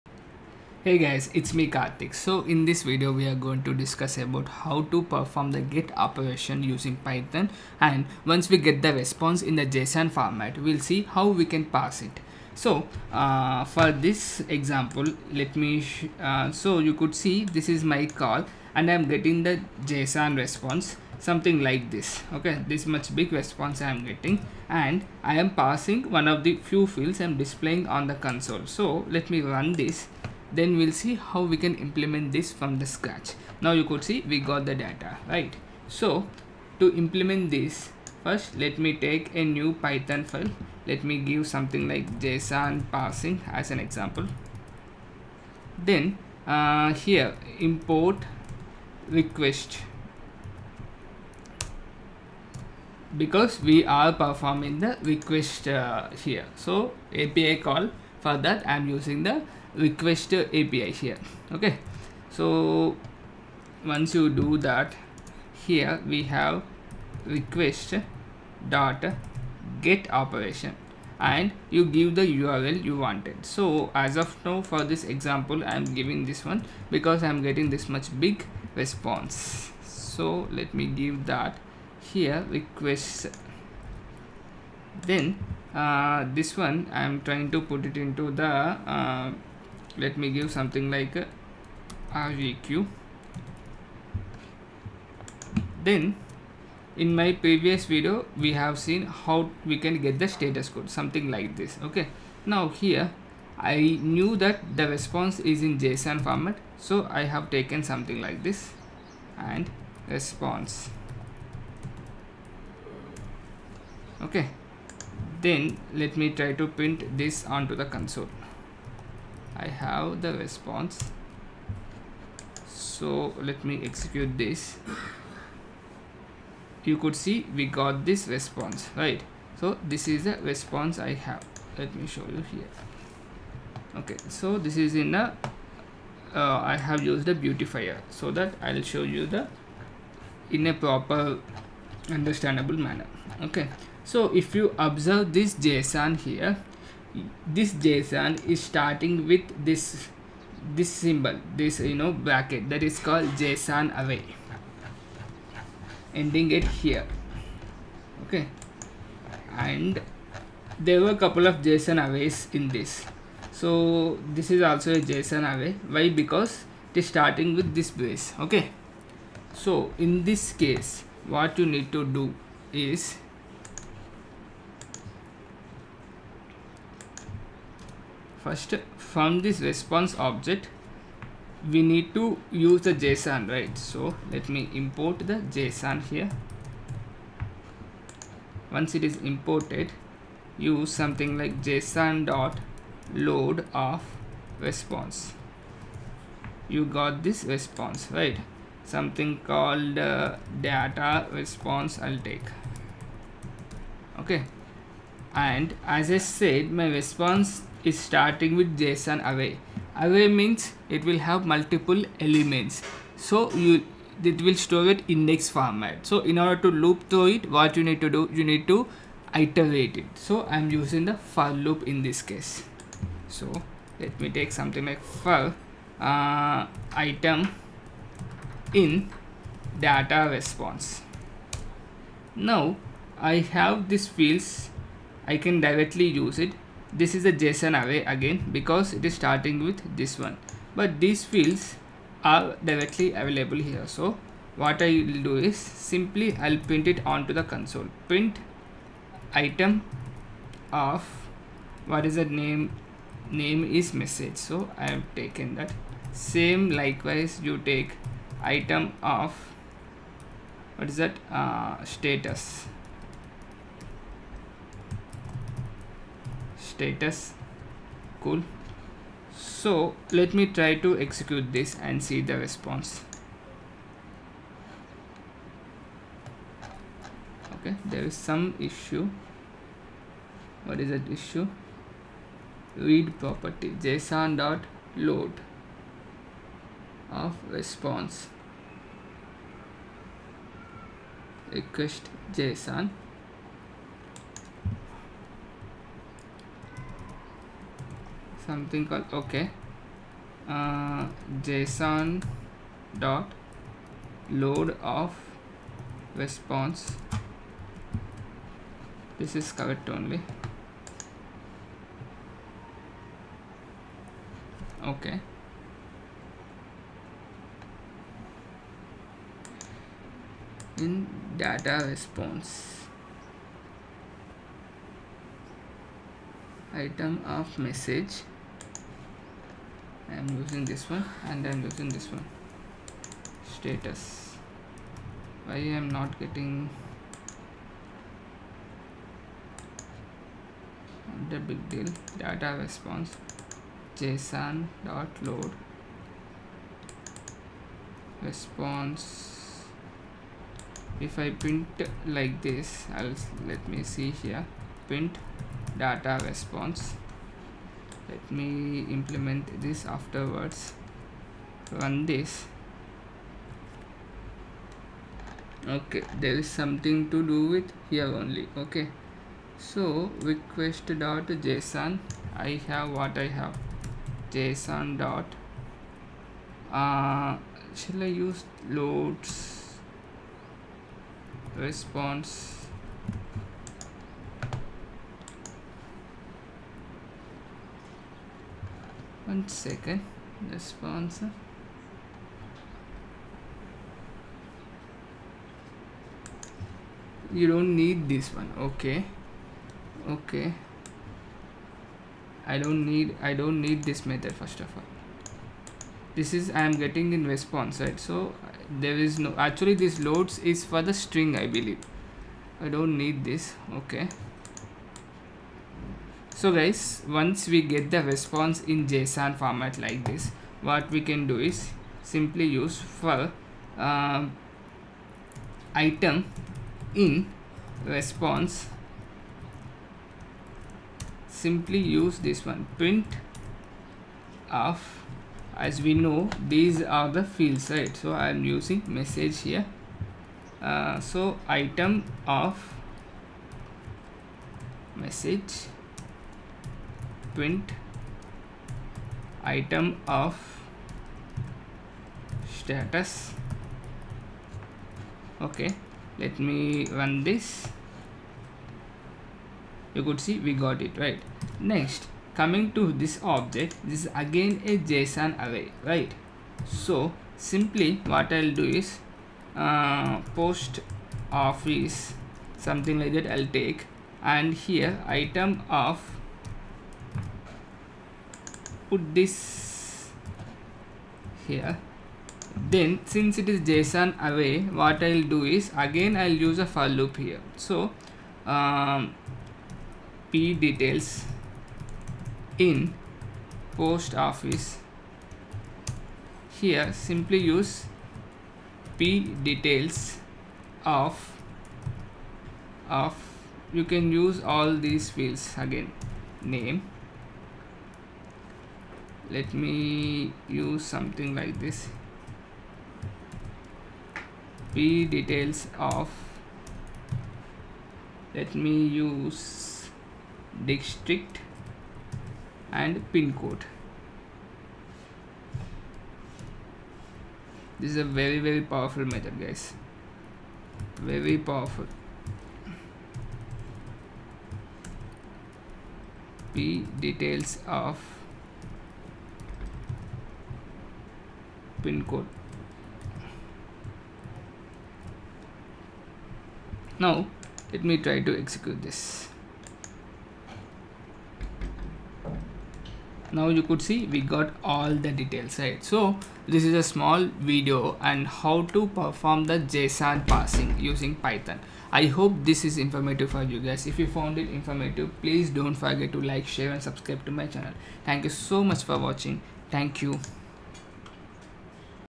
Json Parsing In Python || Iterate Over Json Array in python || Json Library in Python
Json parsing in python using json library which is one of in built libraries in python.
Json library allows us to parse json objects.For json array parsing in python we need to use for loop to iterate over list of keys and values in json.